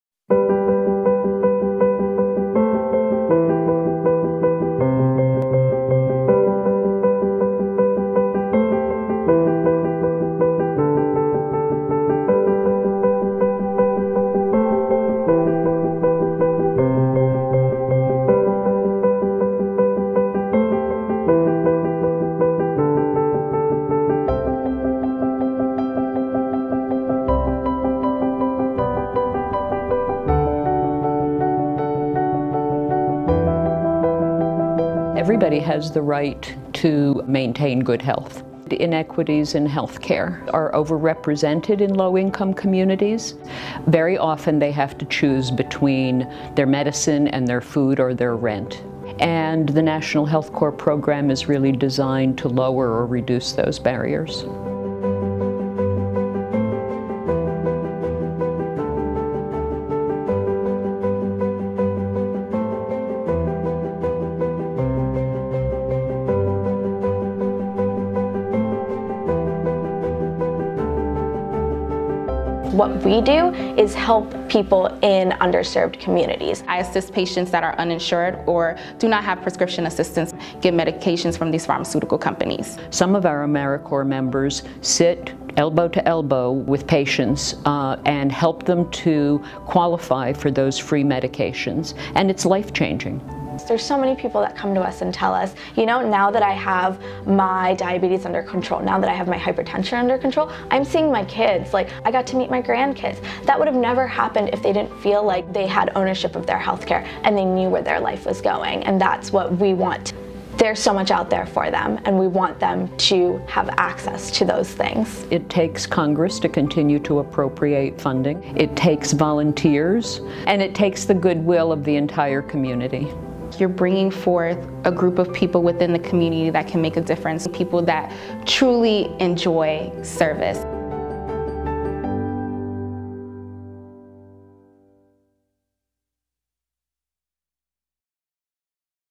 National Health Corps: Our Mission
National Health Corps connects people who need it most with health education and services today, while developing tomorrow’s compassionate health leaders.